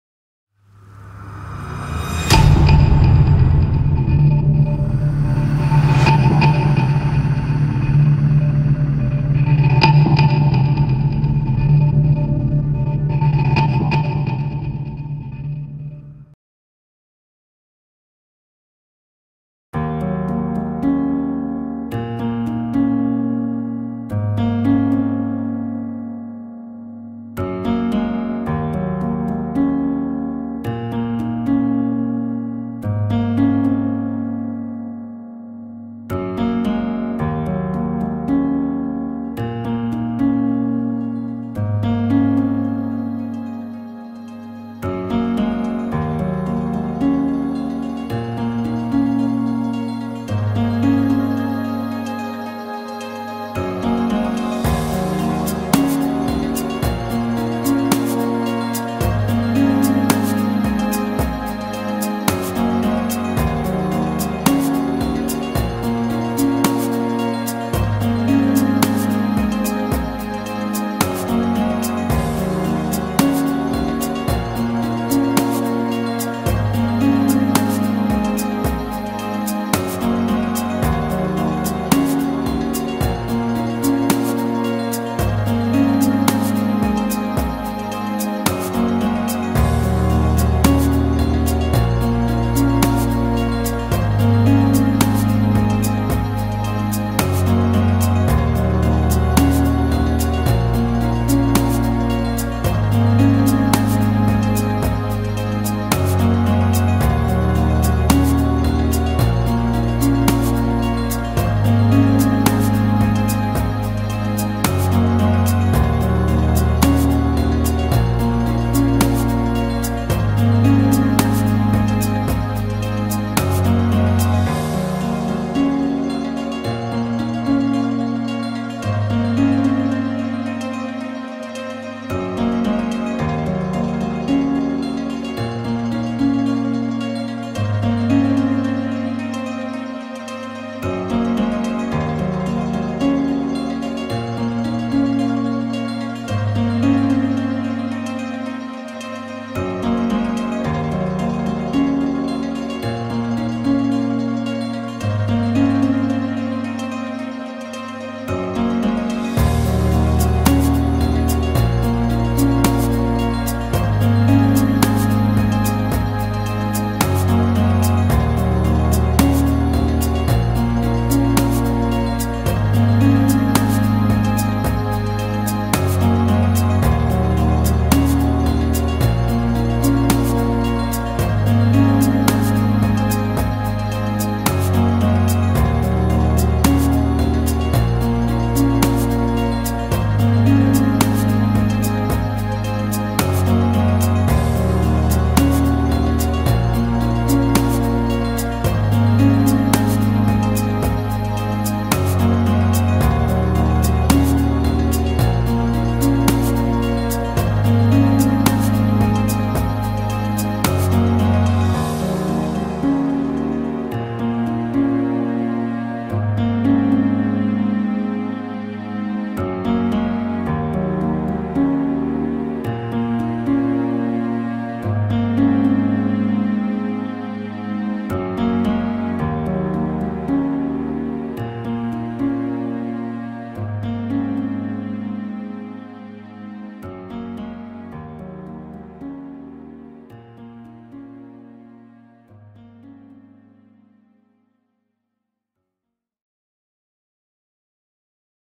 4K / Leiser Berge / Weinviertel / Niederösterreich / Austria
Dieses Video ist ein Test für ein Projekt über das Weinviertel.

Flugaufnahmen: Yuneec Typhoon H

The track is available for non-commercial usage completely free of charge, falling under the Creative Commons NonCommercial License.

00:00 - 00:19   Intro
00:19 - 00:54   Galgenberg in Wildendürnbach
00:54 - 01:52   Kunstfeld zwischen Hetzmannsdorf und Karnabrunn
01:52 - 02:27   Herzerlteich bei Karnabrunn
02:27 - 02:51   vermutlich ehem. Presshaus bei Hipples
02:51 - 03:33   Schloss Ernstbrunn mit Schüttkasten und Tierpark Dörfles
03:33 - 04:09   Mohnfeld vor Kleinebersdorf